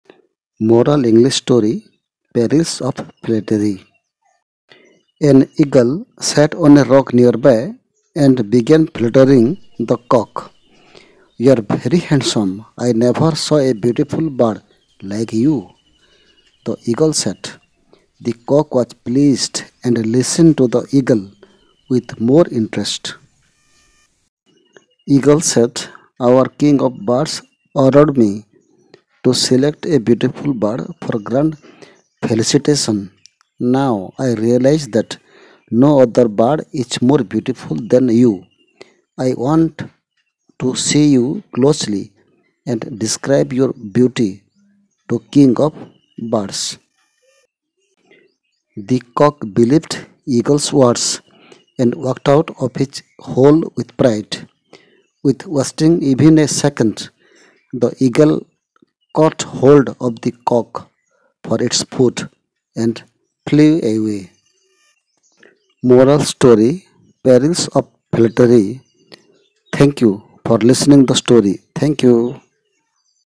Perils of flattery. Tales in English. Moral English story for small children and teenagers.
This moral story is based on panchatantra stories for small children and teenagers.